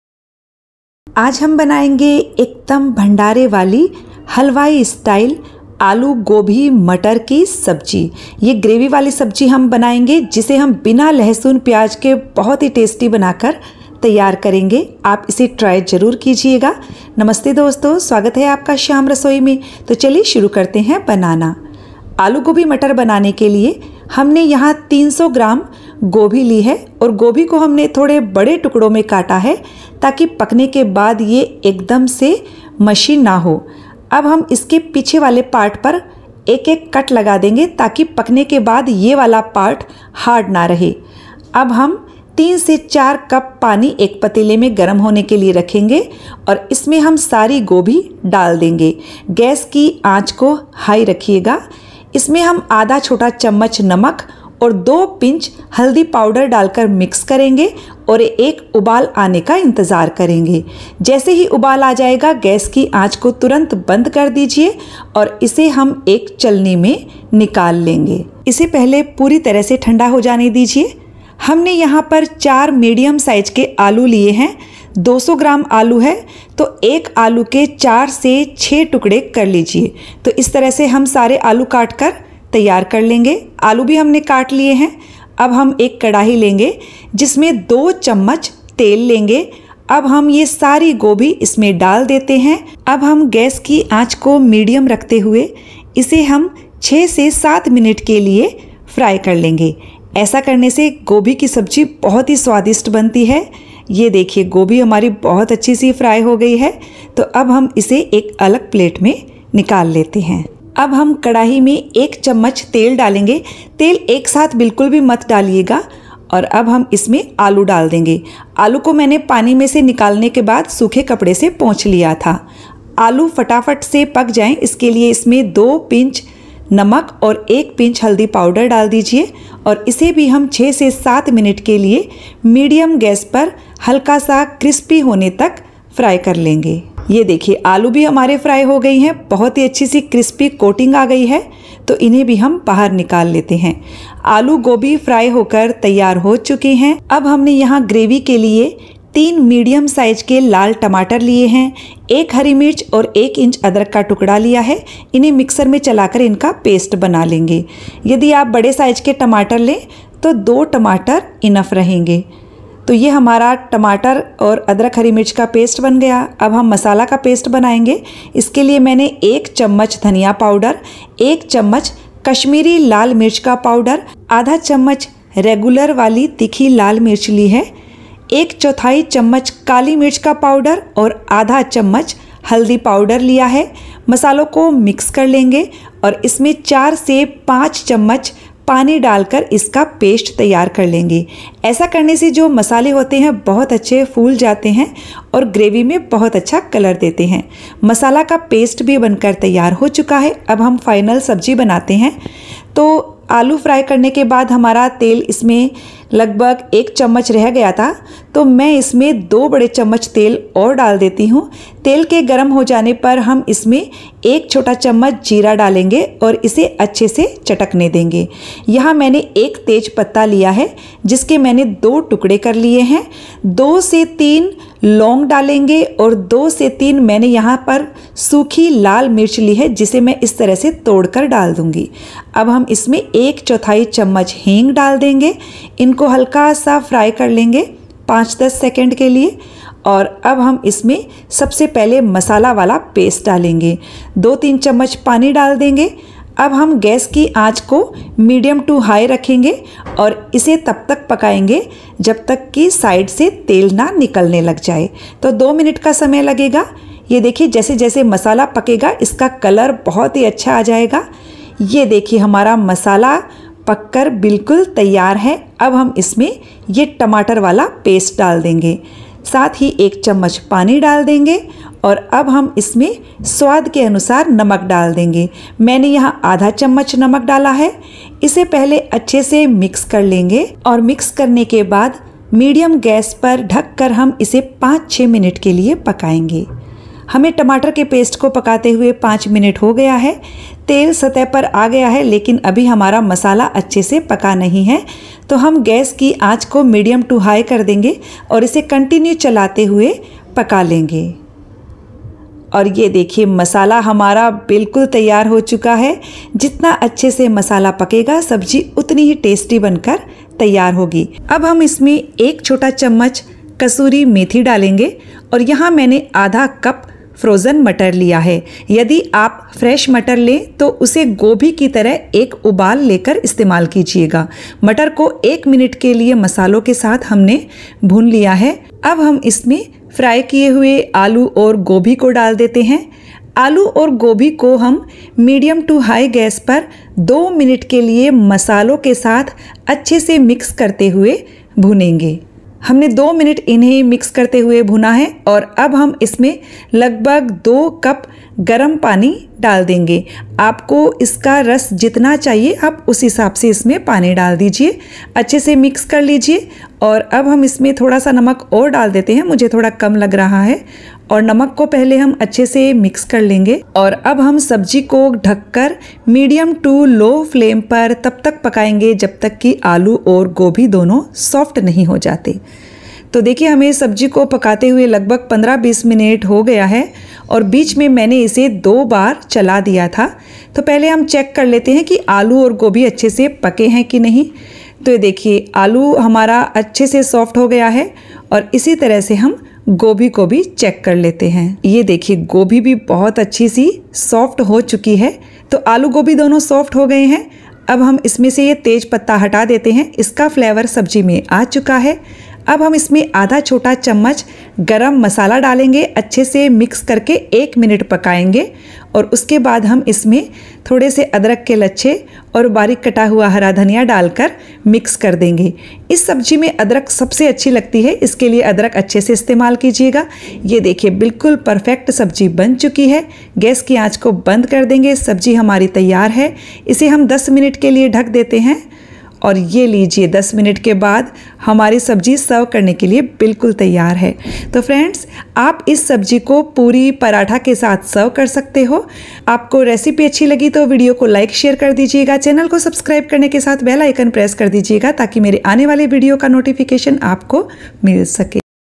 हलवाई स्टाइल आलू गोभी मटर की मसालेदार सब्जी। Aloo Gobi Recipe। Bina lahsoon pyaz aloo gobhi recipe।
हलवाई स्टाइल आलू गोभी मटर की सब्जी बनाने की विधि। Aloo Gobi Recipe।Gobhi ki Sabzi। Aloo Gobhi @ShyamRasoi

Ingredients List-
300 gm Cauliflower
4 medium Potatoes
2 tbsp + 1 tbsp + 2 tbsp Oil
3 Tomatoes
1 Green Chilli
1 inch Ginger
1 Spoon Coriander Powder
1 Spoon Kashmiri Red chilli powder
1/2 Spoon Red Chilli Powder
1/4 Spoon Black Pepper powder
1/2 Spoon Turmeric Powder
1 tsp Cumin Seeds
1 Bay leaf
2-3 Cloves
2-3 Dry Red Chilli
1/4 tsp Asafoetida
1 tsp Kasuri Methi
1/2 Cup Frozen Green Peas
1/2 tsp Garam masala
Ginger Julienne
Coriander leaves

Things I USE IN MY VIDEO:
Steel Kadhai
Dubbing mic
Non- Stick Tava
LG Microwave Oven